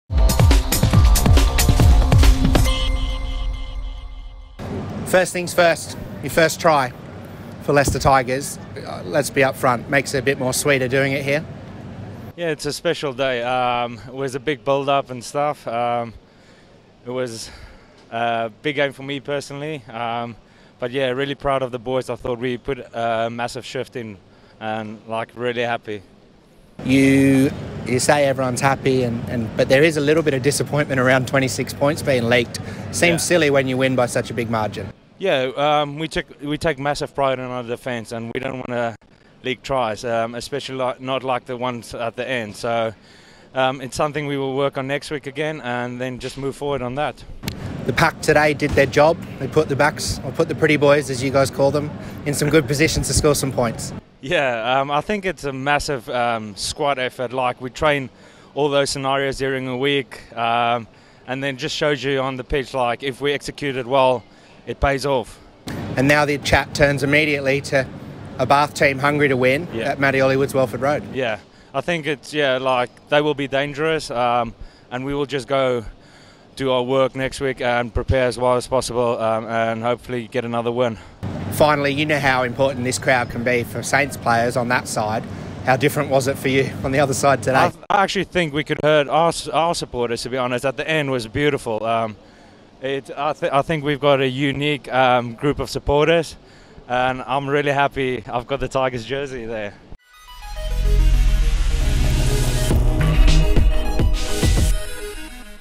Post-Match Chat: Francois van Wyk
Francois van Wyk speaks to LTTV following the away win over Northampton Saints in Round 7 of the 21/22 Gallagher Premiership season.